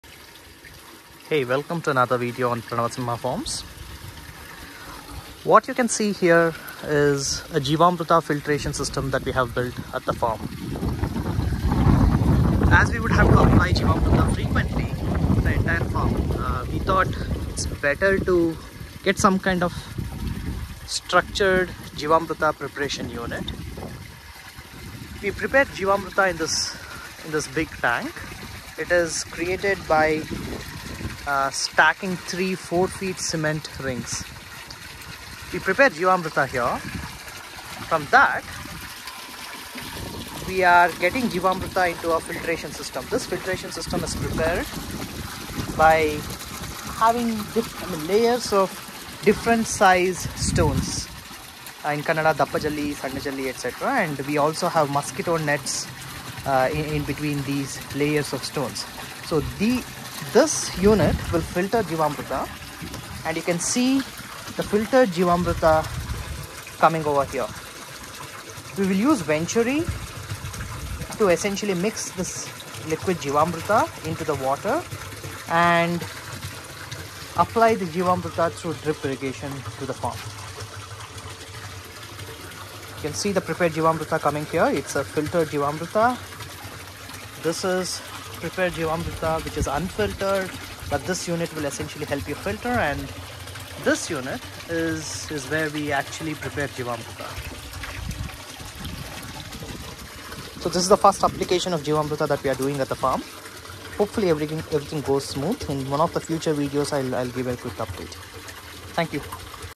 Jeevamrutha Filtration System @PranavaSimhaFarms | Subhash Palekar Natural Farming | Coconut model
I talk about the Jeevamrutha Filtration System that I have implemented at PranavaSimha Farms. I have used cement rings of 4 feet (for Jeevamritha preparation unit) and 3 feet (for the filter and delivery unit).